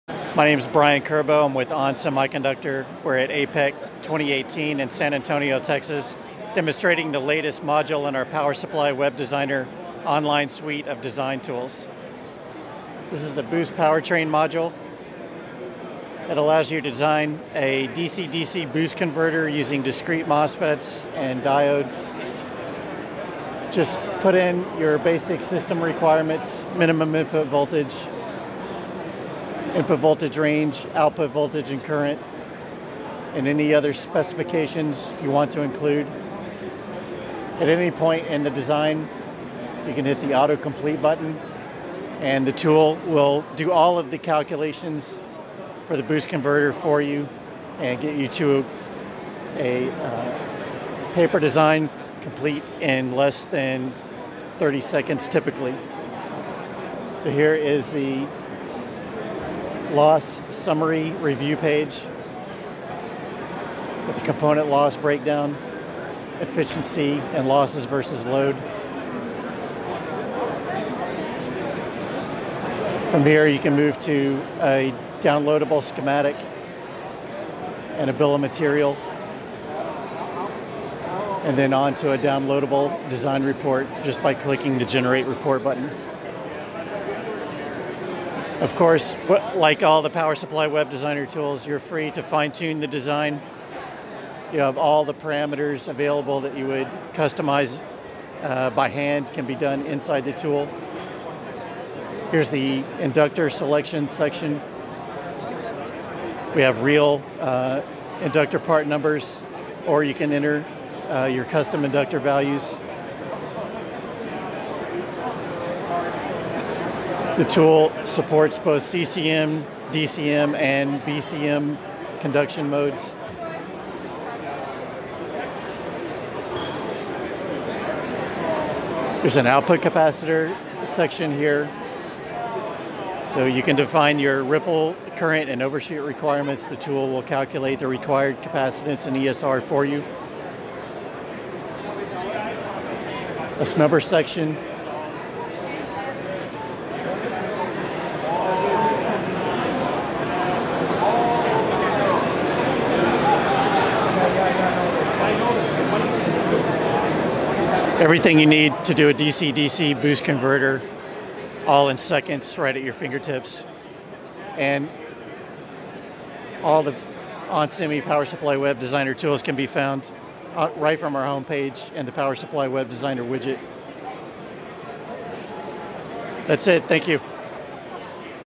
OnSemi demonstrates their Power Supply WebDesigner software tool
In this video Brian from On Semiconductor explains the latest module in their  Power Supply WebDesigner online tool at APEC 2018 in San Antonio, Texas. The new module allows you to design a DC-DC boost converter using discrete components.